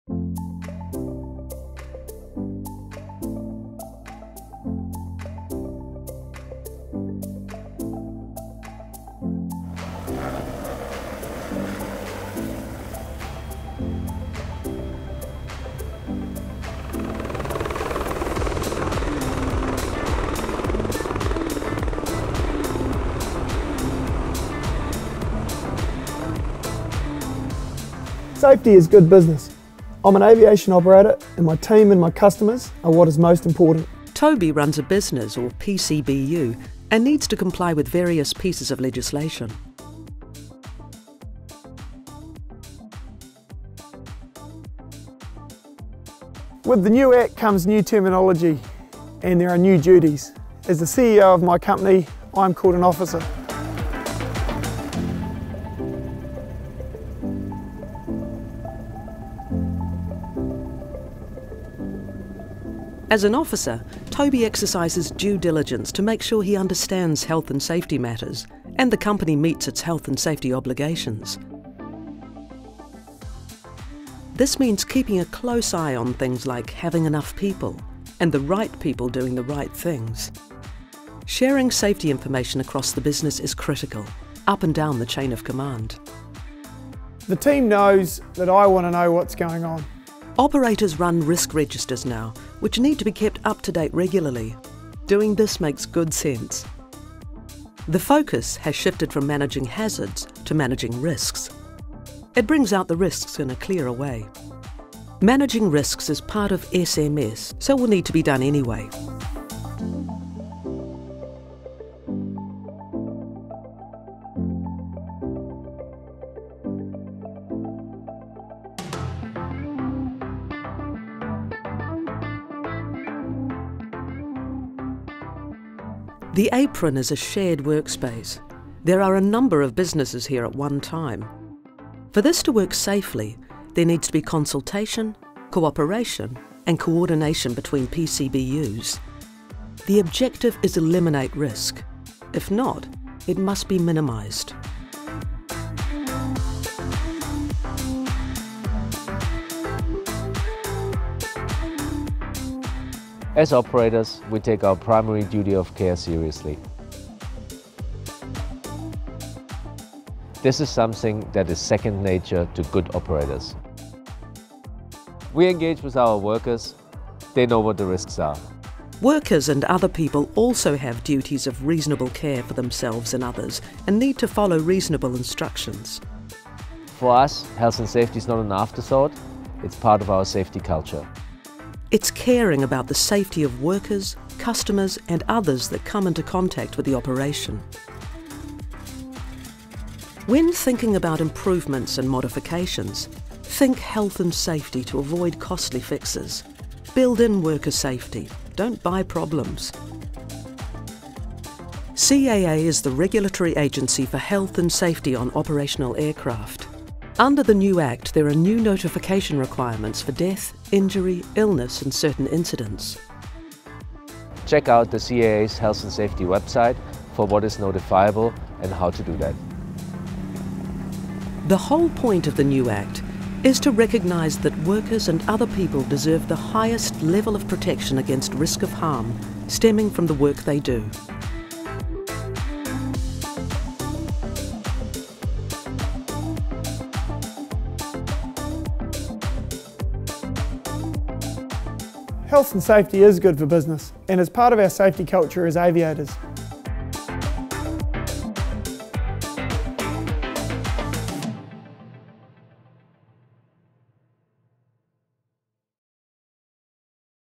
CAA - Health and Safety is Second Nature
Health and Safety is second nature. This video has been prepared by the Civil Aviation Authority of NZ Health and Safety Unit.
It is for commercial aviation operators, featuring operators to  better understand the Health and Safety at Work Act 2015 and the obligations of duty holders.